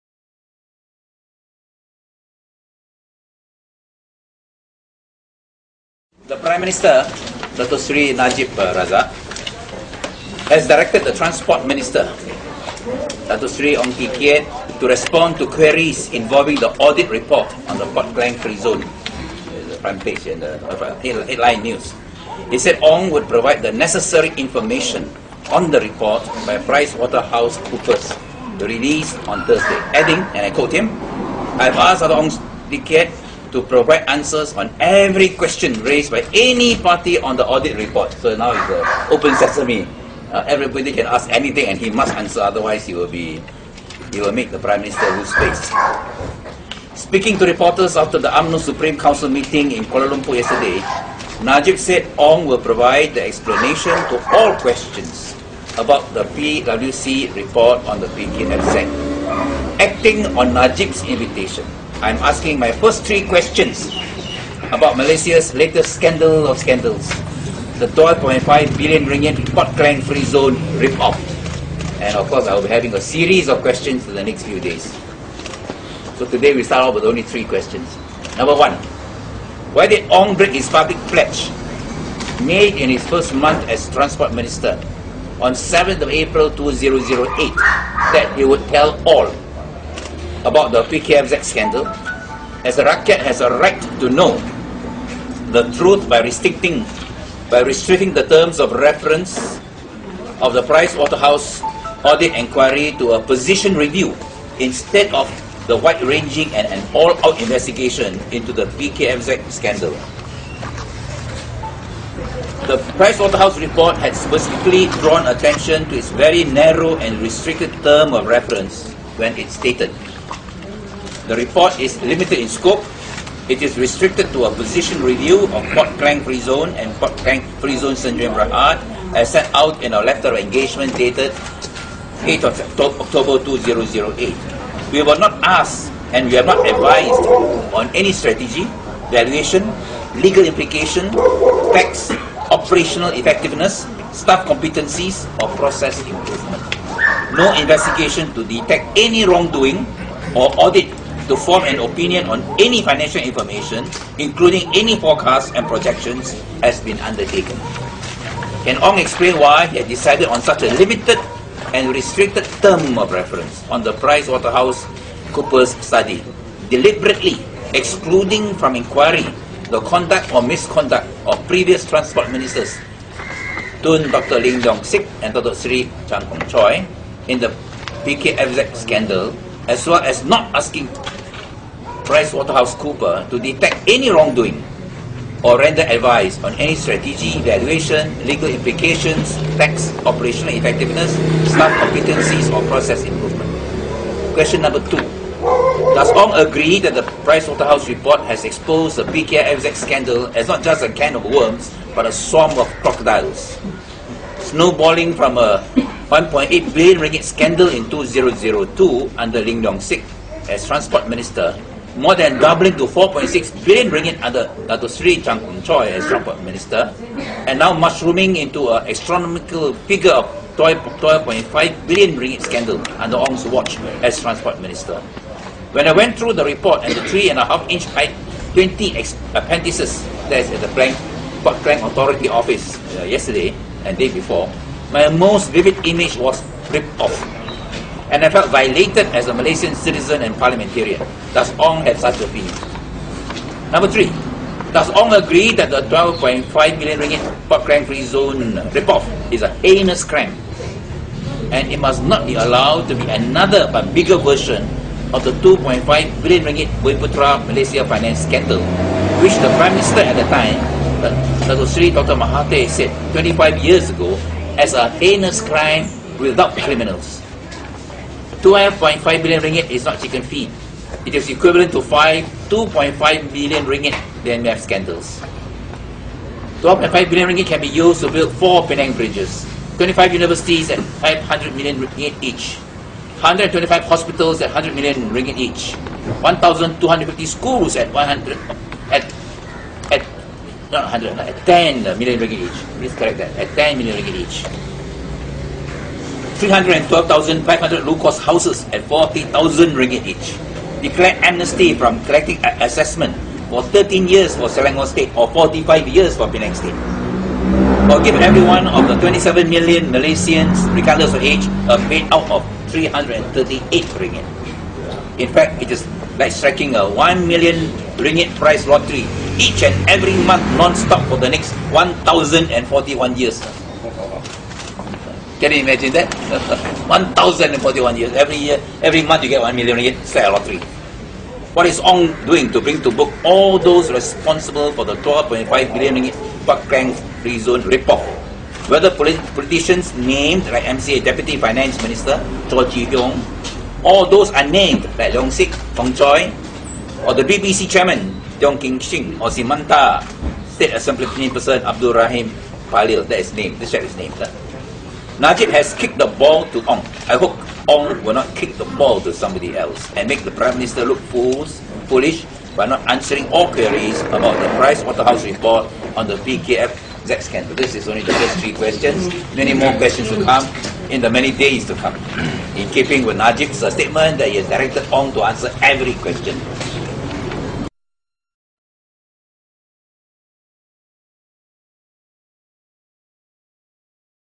Kit Siang: PKFZ rip-off - Malaysia News
Malaysia News

Lim Kit Siang described the PKFZ as a rip-off and gave the following to illustrates theRM12.5 billion.

RM12.5 billion is not chicken-feed. It is equivalent to five RM2.5 billion BMF scandals. RM12.5 billion can be used to build four Penang bridges, 25 universities at RM500 million each; 125 hospitals at RM100 million each; 1,250 schools at RM10 million each; 312,500 low-cost houses at RM40,000 each; declare amnesty from collecting assessment for 13 years for Selangor state or 45 years for Penang state; or give everyone of the 27 million Malaysians regardless of age a payout of RM338!

In fact it is like striking a RM1 Million price lottery each and every month non stop for the next 1,041 years!

sumber: cj.my